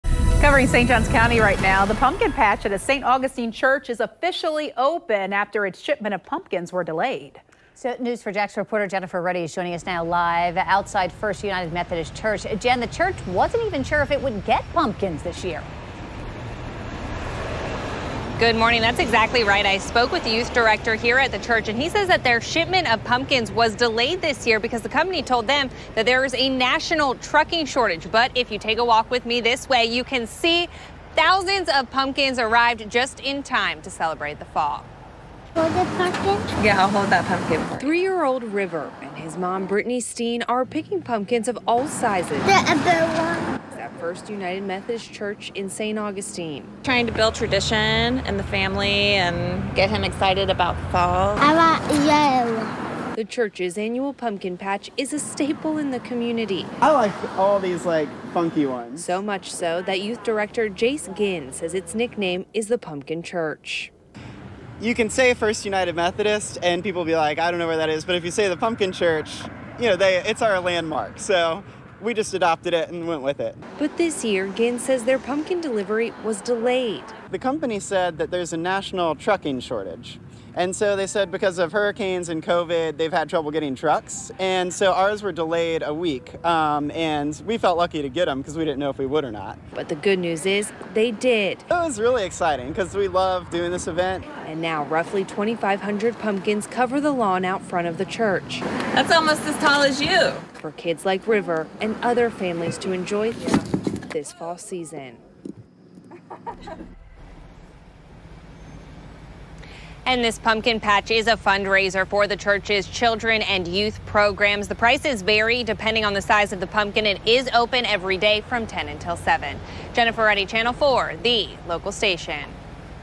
Popular Pumpkin Patch Opens After Shipment Delay
The pumpkin patch at a St. Augustine Church is officially open after its shipment of pumpkins was delayed. News4Jax reporter Jennifer Ready joins us live at First United Methodist Church.